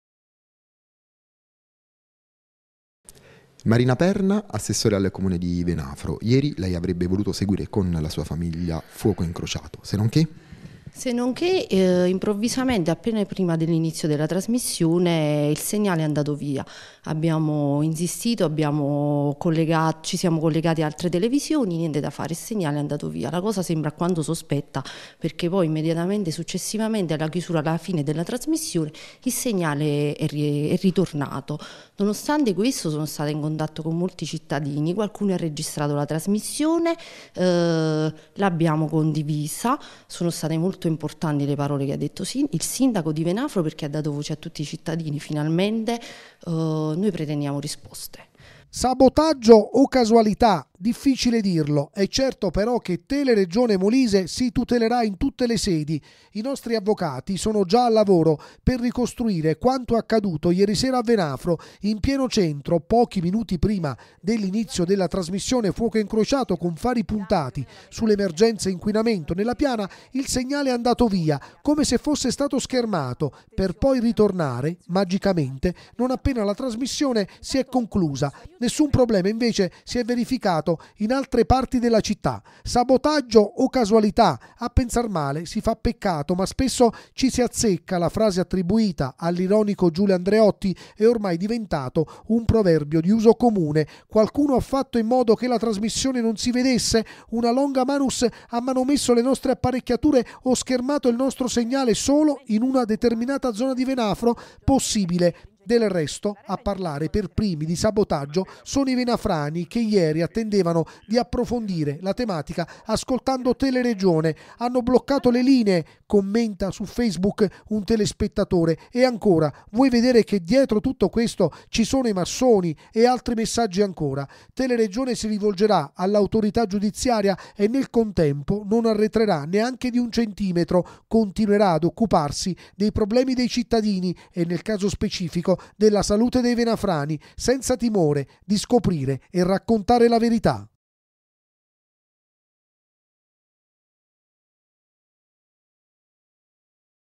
Inquinamento a Venafro, Teleregione Molise approfondisce e il segnale sparisce: ...
Servizi TG | Inquinamento a Venafro, Teleregione Molise approfondisce e il segnale sparisce: sabotaggio o casualità? | 14/1/2020